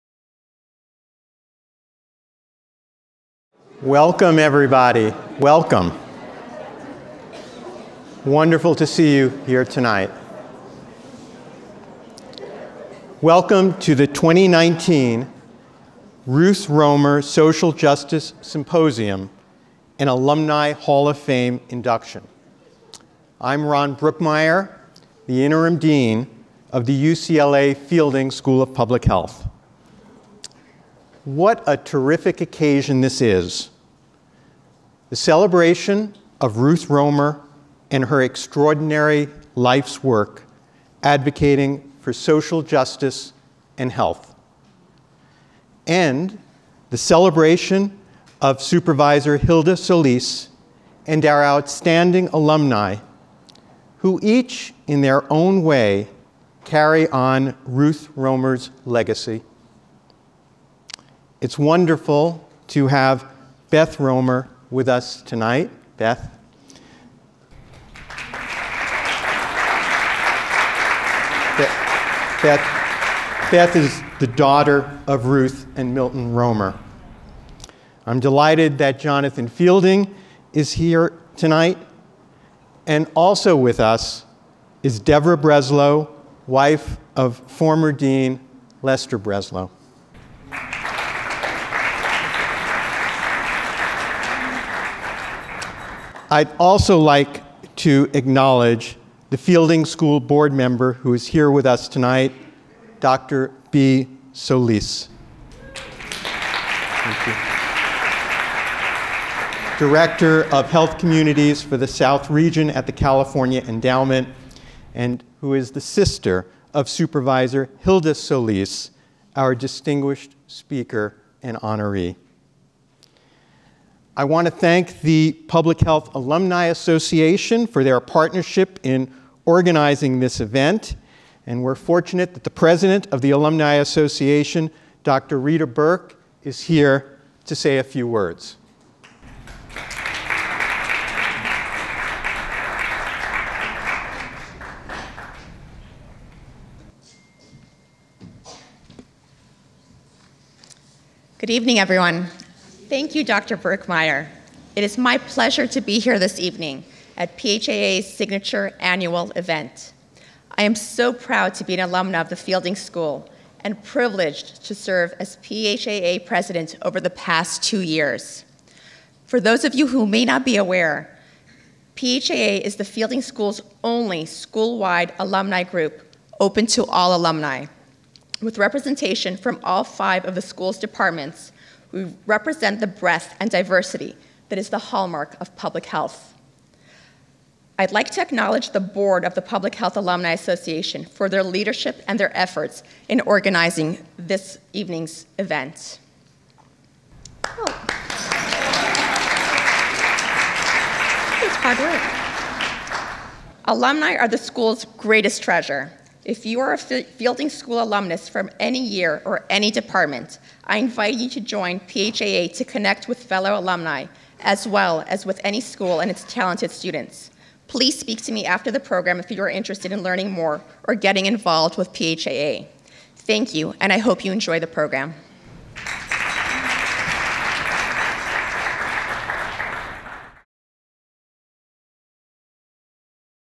4-24-2019 | Ruth Roemer Social Justice Symposium & Alumni Hall of Fame Induction - Introduction
The 2019 Ruth Roemer Social Justice Symposium honored Supervisor Hilda L. Solis, Los Angeles County Supervisor, First District with the Ruth Roemer Social Justice Leadership Award, followed by the FSPH Alumni Hall of Fame induction of Ludlow B. Creary, MD, MPH ’73, FAAFP; Sander Greenland, DrPH ’78, MPH ’76; and Goleen Samari, PhD ’15, MPH ’10, MA ’10.